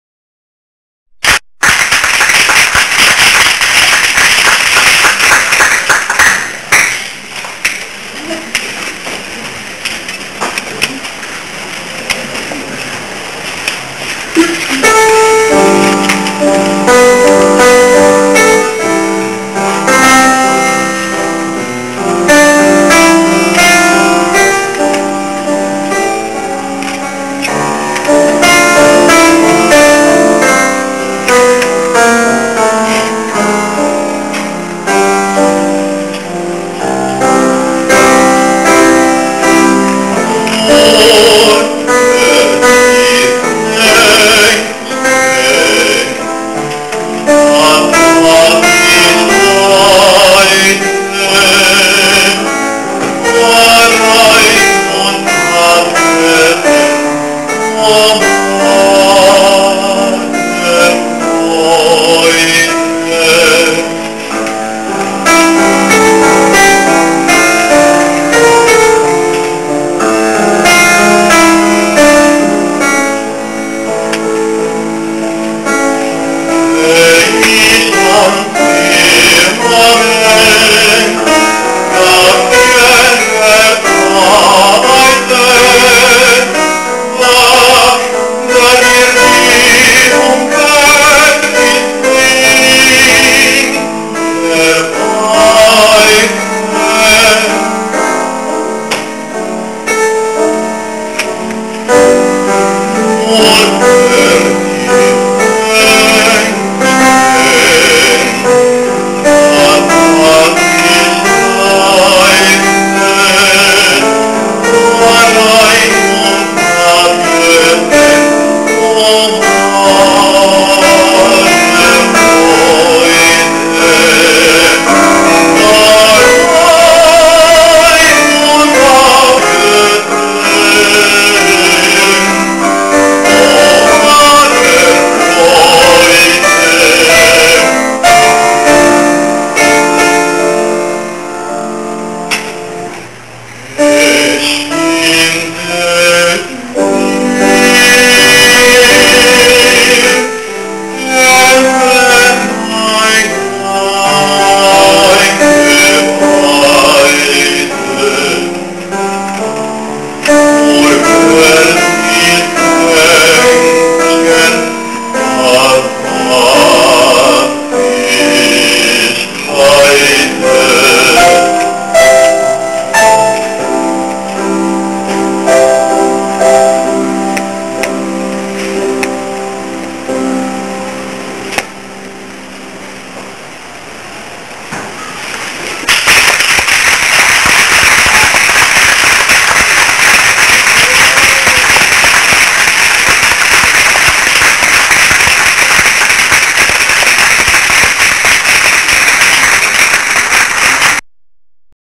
JOHNNY DANIEL ALFARO PEREZ
JOHNNY DANIEL ALFARO PEREZ, UCR, RECITAL DE HONOR, 25 DE JUNIO 2008